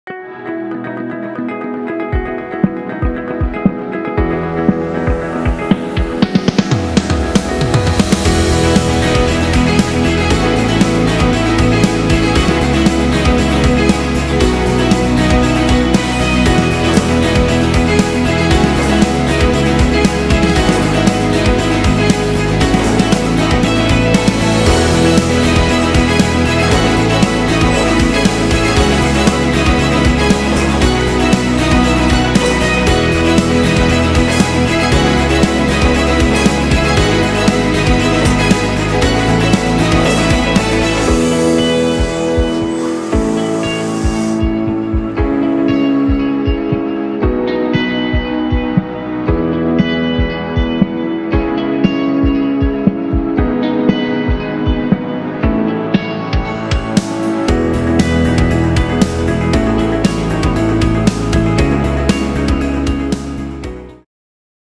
PrimaProductPills - How to make high upformings on sheet metal thanks to Prima Power Punch Sharp
PrimaProductPills - How to make high upformings to the sheet with Punch Sharp turret punch press. Louvers, knockouts, extrusions - up to 16 mm height.